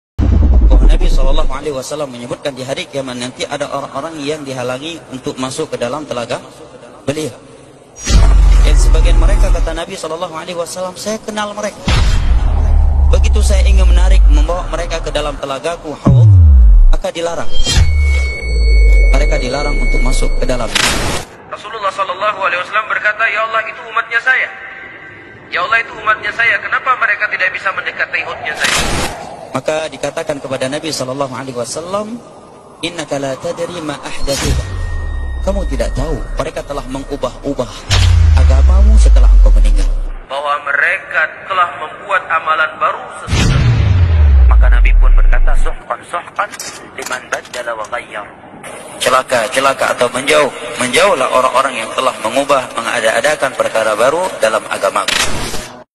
UMAT ISLAM YANG DI USIR DARI TELAGA SURGA RASULULLAH
UMAT ISLAM YANG DI USIR DARI TELAGA RASULULLAH,

Tentang keberadaan haudh (telaga) di hari ini, telah dijelaskan oleh Nabi shallallahu 'alaihi wa sallam dalam banyak hadis dengan redaksi yang berbeda.

TIDAK ADA ALASAN LAIN MEMBUAT KONTEN Atau VIDEO Kajian SEPERTI INI Hanya ingin berusaha berbuat amal sholeh...

. “Jika anak Adam meninggal, maka amalnya terputus kecuali dari tiga perkara, sedekah jariyah (wakaf), ilmu yang bermanfaat, dan anak shaleh yang berdoa kepadanya.” (HR Muslim).

 Jazakumullah Khairan Sudah menonton,
Barakallah fiikum.